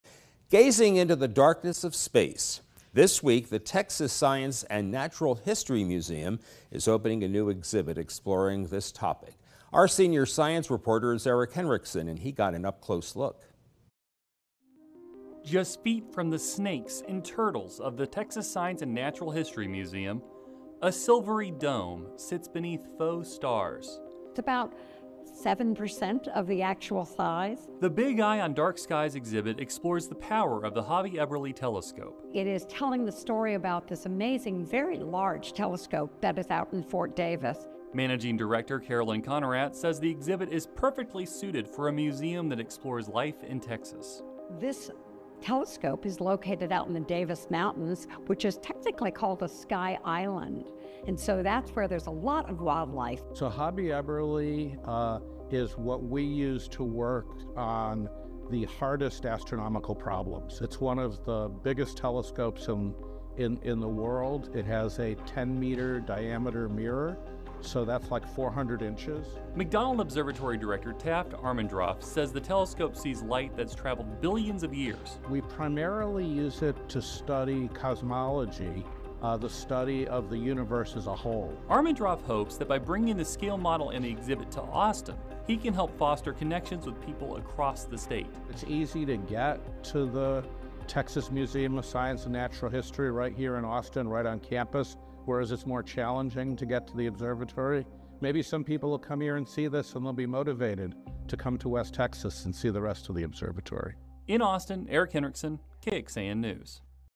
McDonald Observatory relocates to Austin — at 7% the size
The Texas Science and Natural History museum has opened a new exhibit, "Big Eye on Dark Skies," which features a scale model of the Hobby-Eberly telescope, one of the world's most powerful telescopes. The exhibit is designed to showcase the telescope's capabilities, which include collecting light from eleven billion years ago and studying the universe as a whole. The exhibit is located on the Texas Wildlife floor of the museum and is meant to inspire visitors to visit the real Hobey-Eberly and the McDonald Observatory in West Texas.